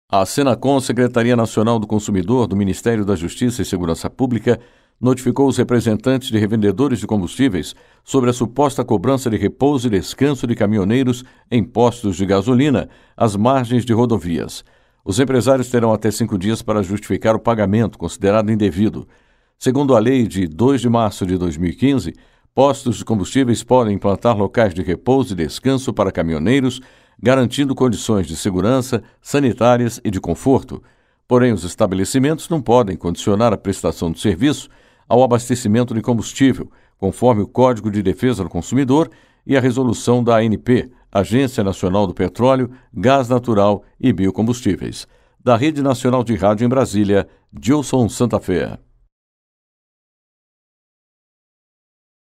10/02/2022 - SENACON NOTIFICA REVENDEDORAS DE COMBUSTÍVEIS
A notificação é sobre a suposta cobrança de repouso e descanso de caminhoneiros em postos de gasolina às margens de rodovias.

A Senacon, Secretaria Nacional do Consumidor, do Ministério da Justiça e Segurança Pública, notificou os representantes de revendedores de combustíveis sobre a suposta cobrança de repouso e descanso de caminhoneiros em postos de gasolina às margens de rodovias. Os empresários terão até 5 dias para justificar o pagamento considerado indevido.

Segundo a lei de 2 de março de 2015, postos de combustíveis podem implantar locais de repouso e descanso para caminhoneiros, garantindo condições de segurança, sanitárias e de conforto. Porém, os estabelecimentos não podem condicionar a prestação do serviço ao abastecimento de combustível, conforme o Código de Defesa do Consumidor e a Resolução da ANP, Agência Nacional do Petróleo, Gás Natural e Biocombustíveis.

registrado em